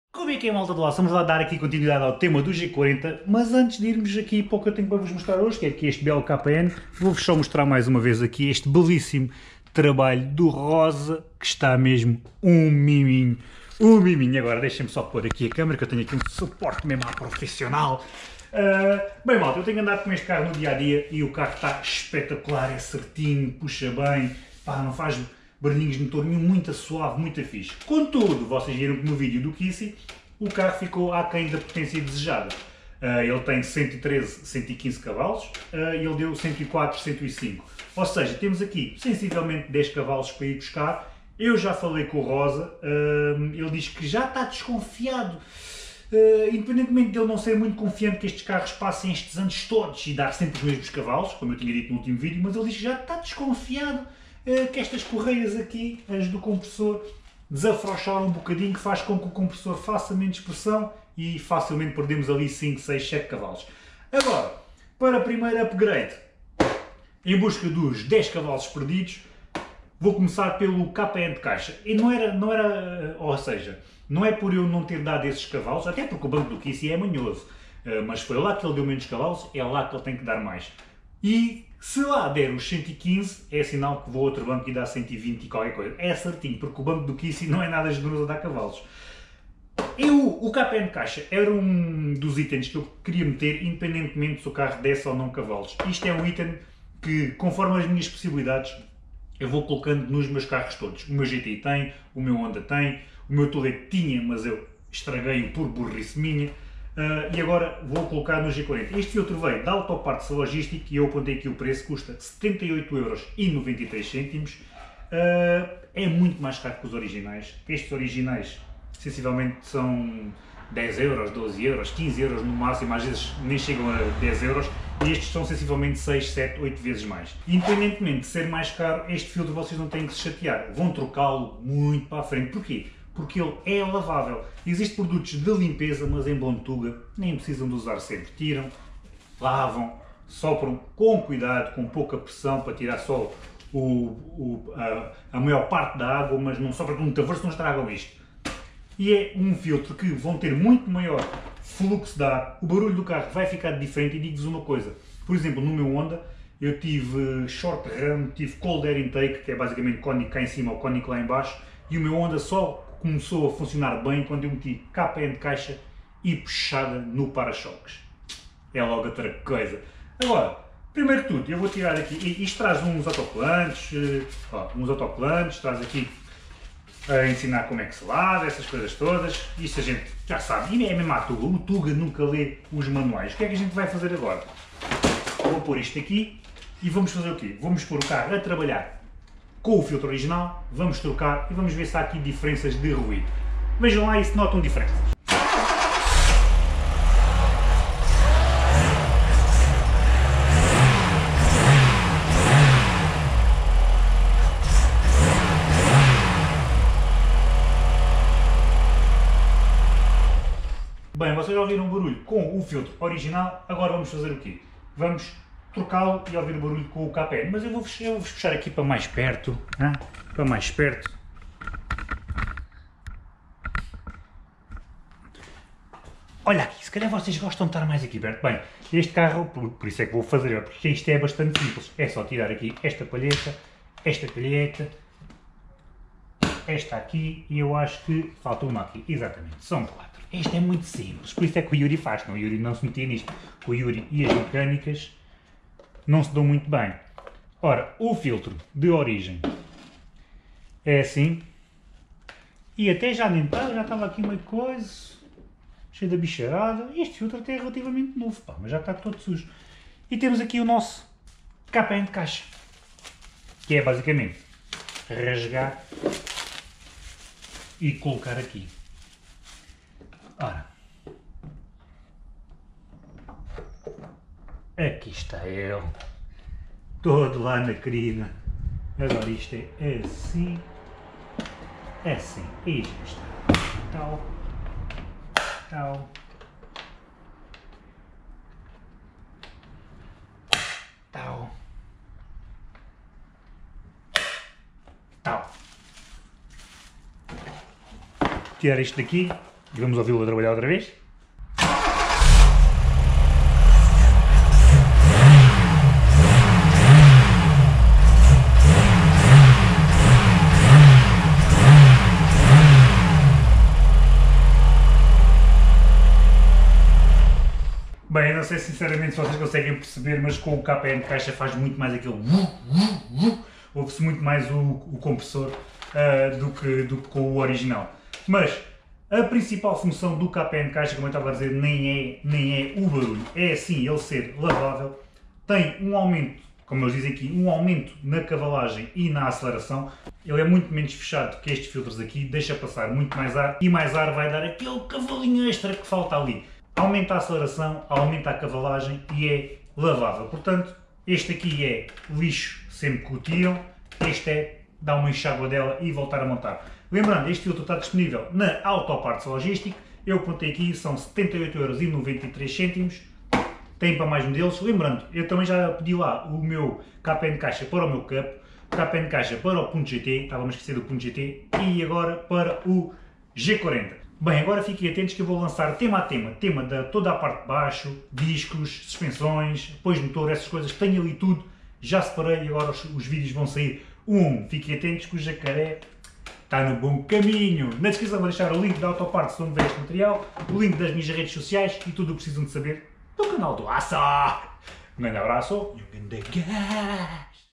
Filtro KN vs Original !!! EM busca dos 10 Cv perdidos no G40
Filtro KN - REF 33-2003